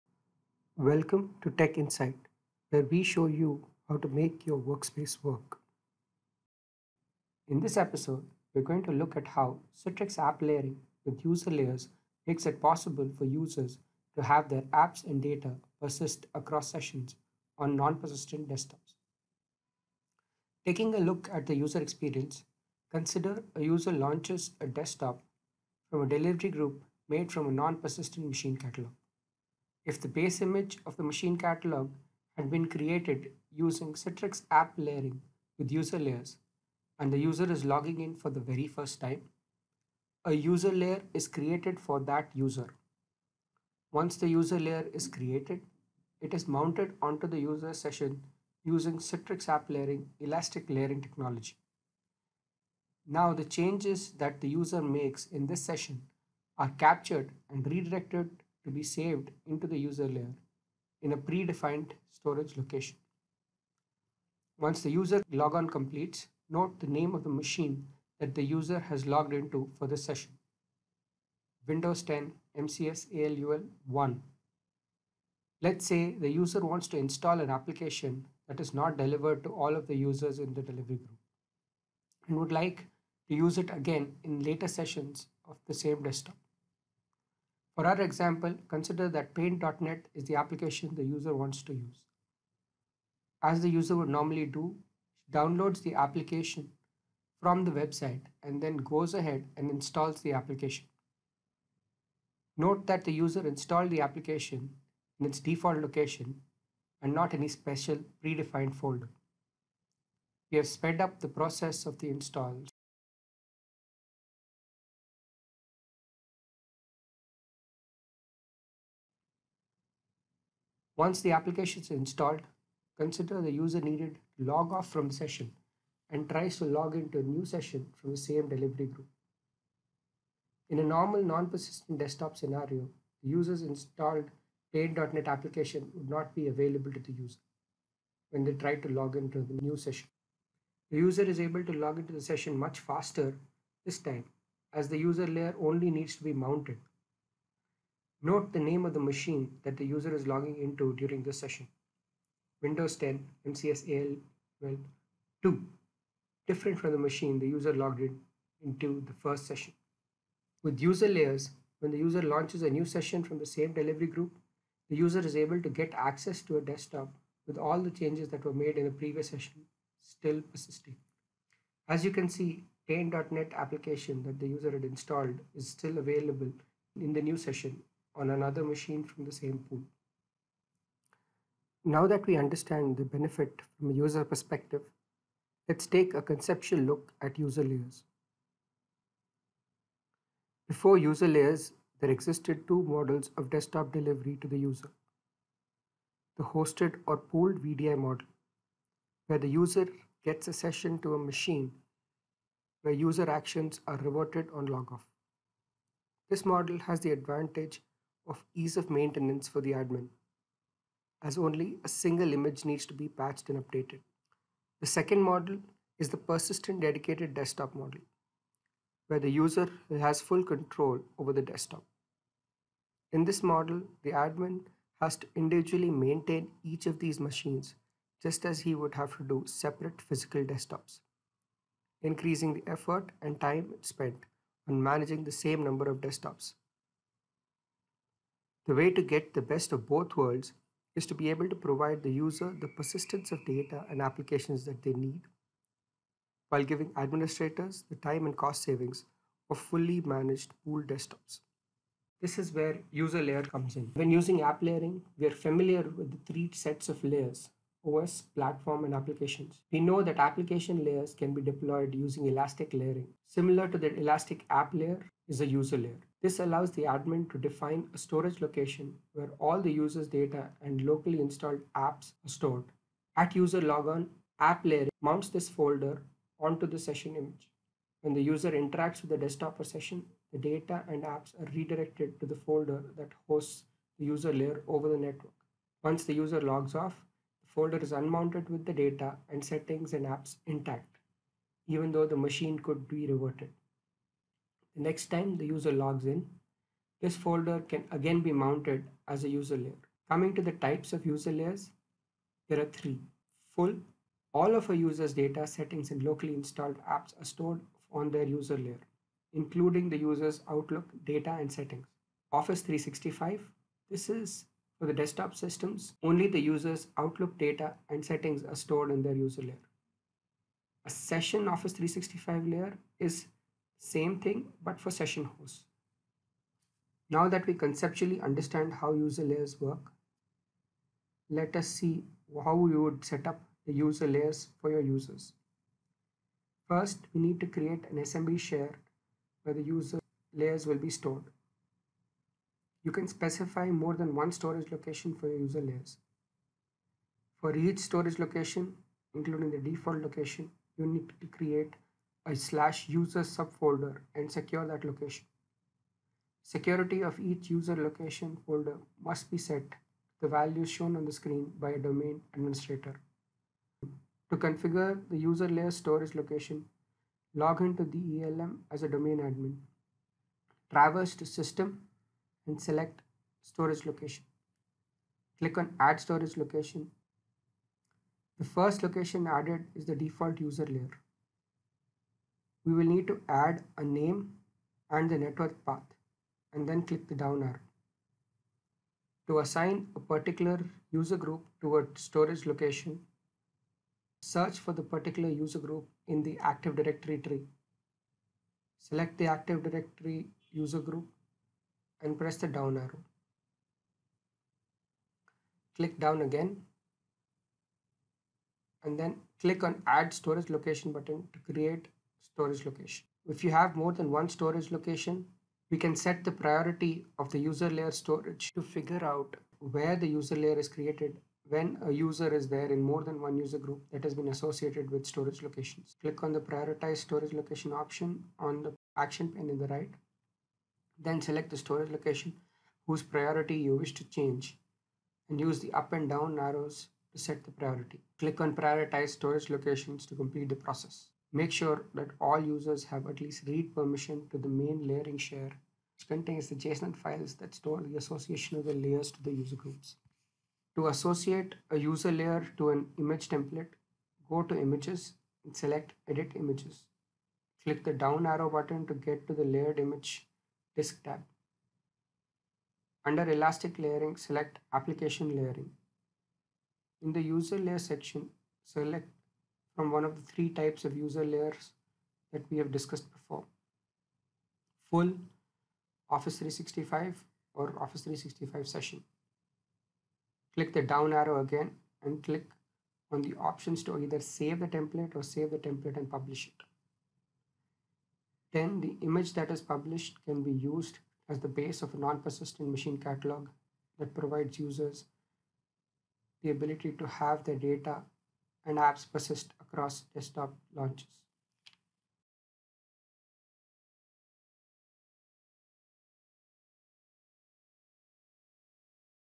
Tech Insight - Citrix App Layering - User Layers
Learn how Citrix App Layering - User layers persist user profile settings, data, and user-installed applications in non-persistent VDI environments.